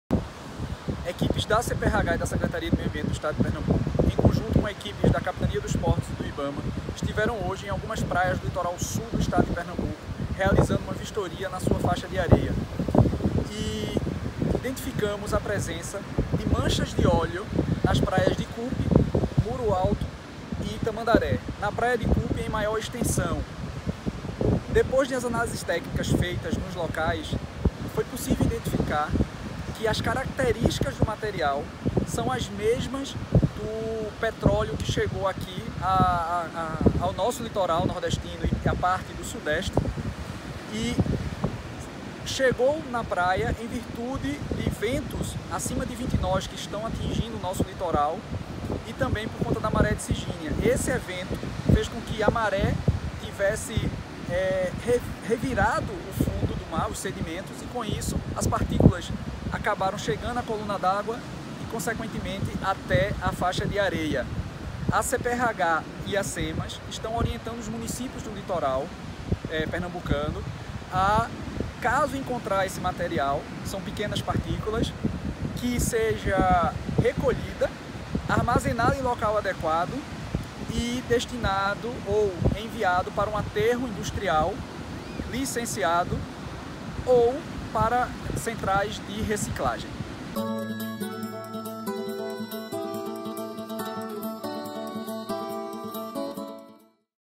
Manchas de petróleo: Eduardo Elvino fala sobre a vistoria nas praias do litoral sul de Pernambuco
Eduardo Elvino, diretor de Controle de Fontes Poluidoras da Agência CPRH, fala sobre o surgimento de pequenos fragmentos de petróleo em praias do litoral sul de Pernambuco. A Semas e a CPRH vistoriaram os locais, junto com representantes da Capitania dos Portos, Ibama e prefeituras.